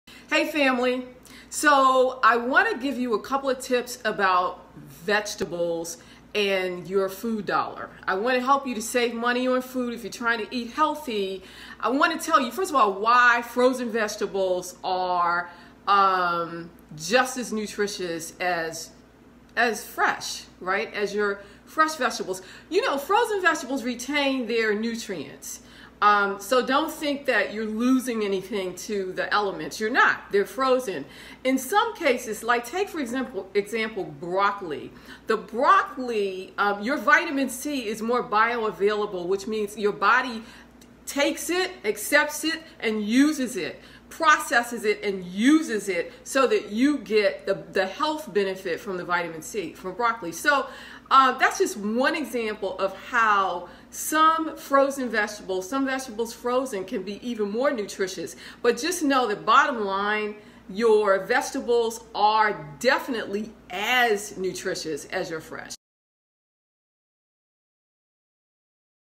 Dr. Ro's Money Saving Tip: Buy Frozen Vegetables
Keep money in your pocket and eat healthy with this tip!

To order Dr. Ro's Lose Your Final 15 book, please